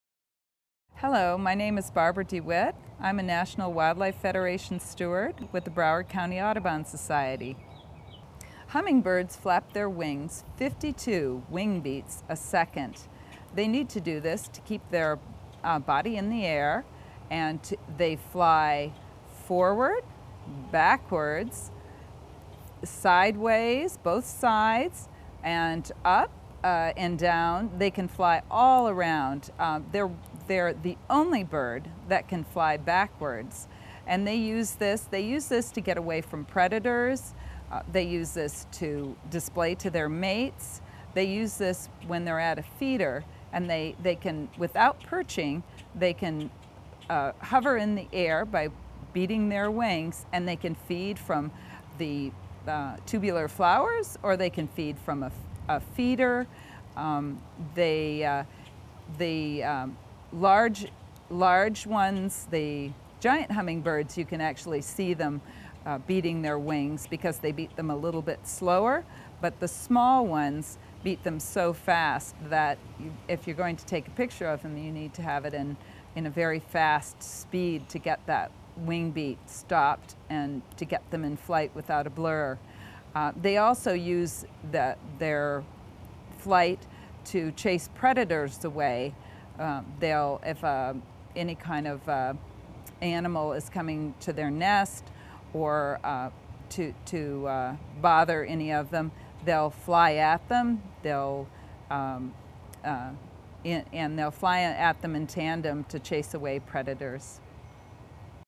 Hummingbirds : How Fast Do Hummingbirds Flap Their Wings?
Hummingbirds can flap their wings as fast as 52 wing beats per second, which enables them to stay in the air flying forward, backwards, sideways, up and down. Discover how hummingbirds get around so quickly with information from a national wildlife steward in this free video on bird life.

Bio: Barbara Ann Dewitt is a national wildlife steward and master gardener who has specialized in butterflies and birds since 1979.